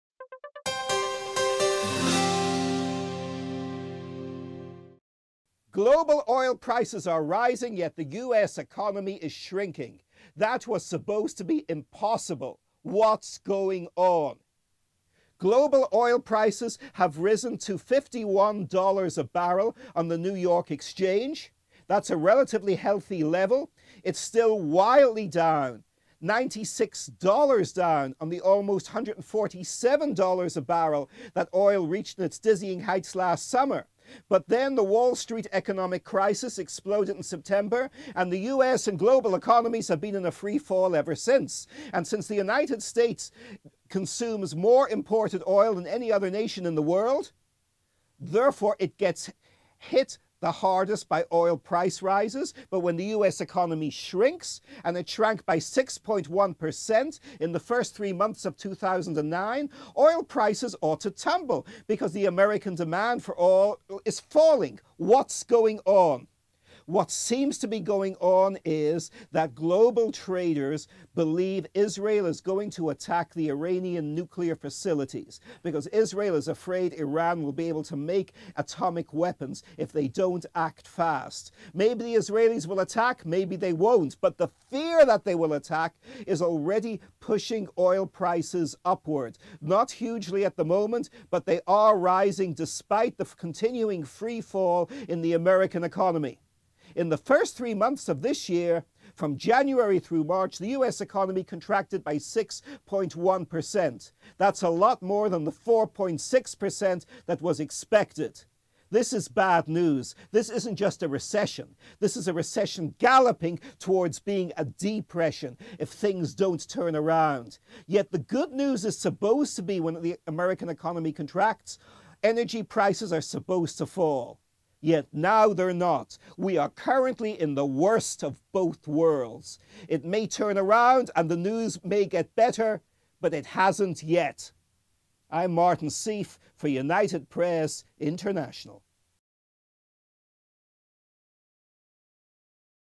Oil prices on the rise
A bi-weekly commentary by UPI writer, Martin Sieff.